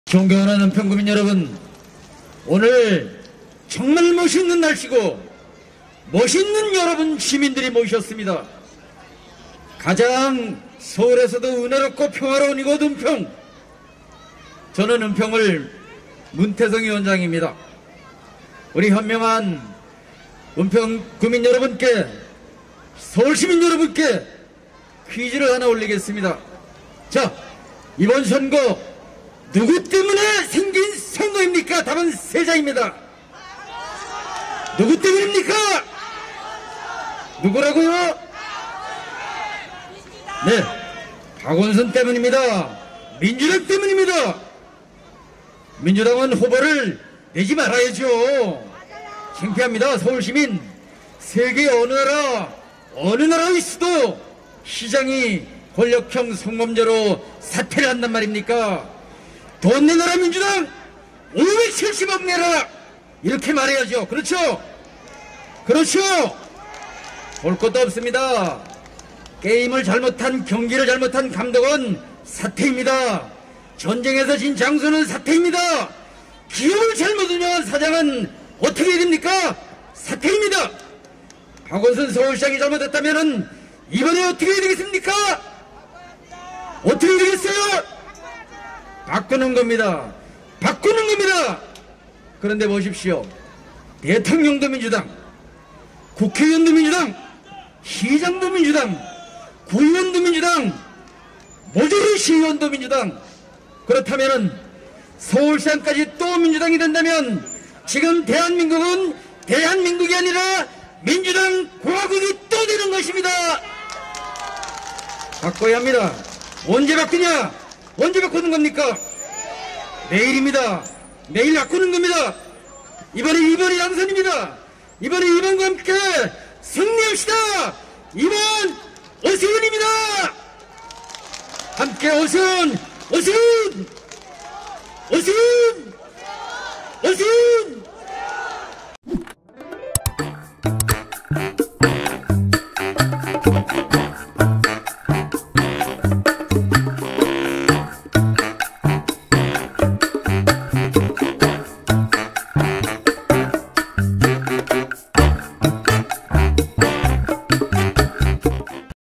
오세훈 지원 사격한 문태성 "박영선 서울시장된다? 그럼 민주당 공화국 되는 것"
21년 4월 6일 열린 국민의힘 은평구 불광천 유세에서 문태성 은평구을 당협위원장이 발언하고 있다.